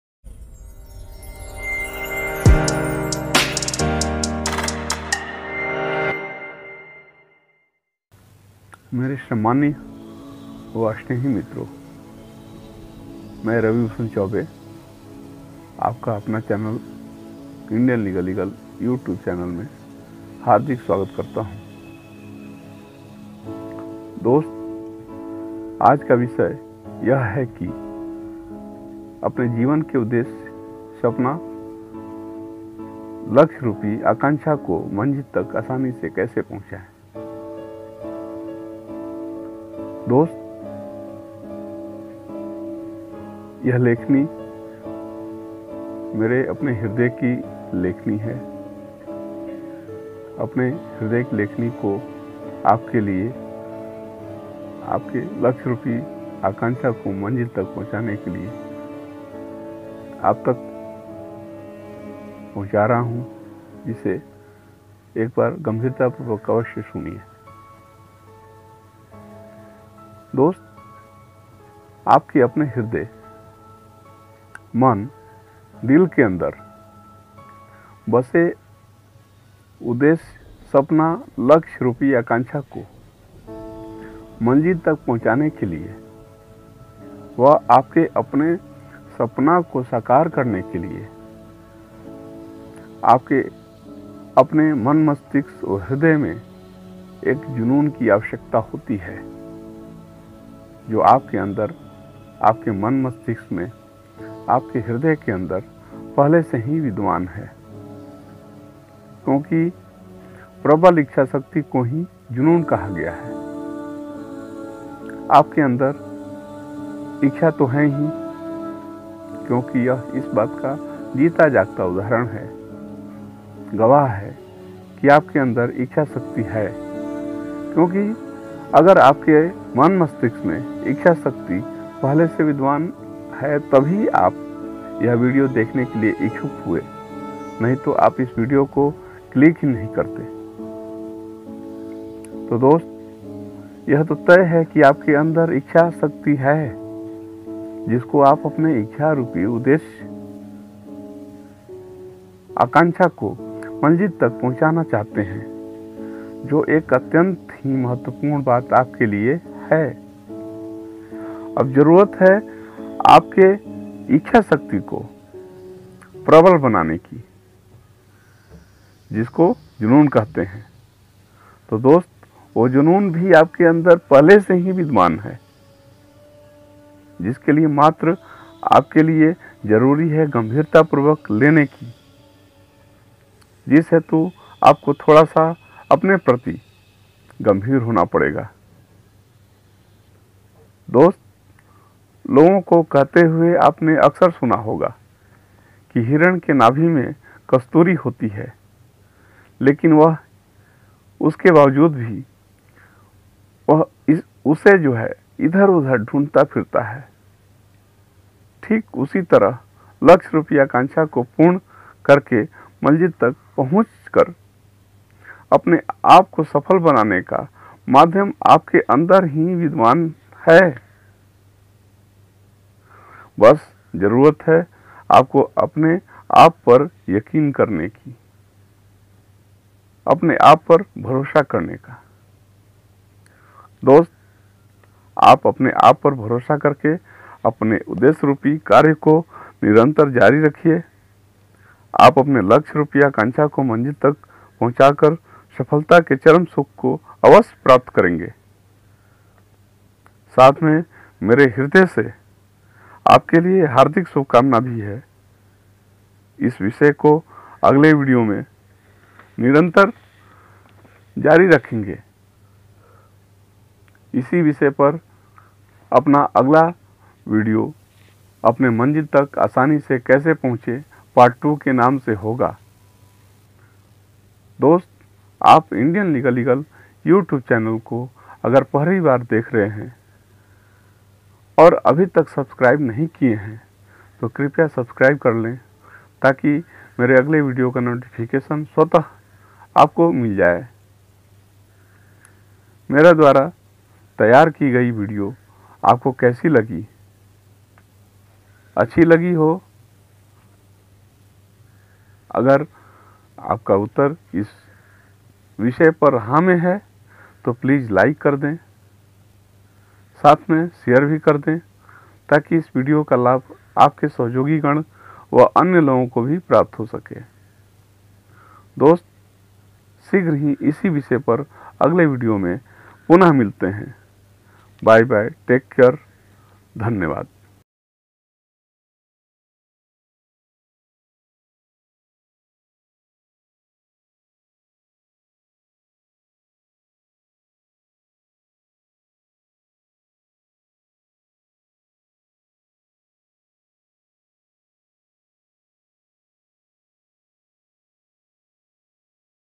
अपने जीवन के लक्ष्य को मंजिल तक आसानी से कैसे पहुंचाएं | Indian Legal Eagle
Apne Jeevan ka Lakshya ko Manjil Tak aasani se kaise

अपने जीवन के लक्ष्य को मंजिल तक आसानी से कैसे पहुंचाएं

आज के इस वीडियो में मैं आपको कुछ ऐसी बातें जिसे आप अपनी जिंदगी जीवन के लक्ष्य को शीघ्र वह आसानी से प्राप्त कर पाएंगे उम्मीद है कि आप वीडियो पूरा देखेंगे
धन्यवाद ।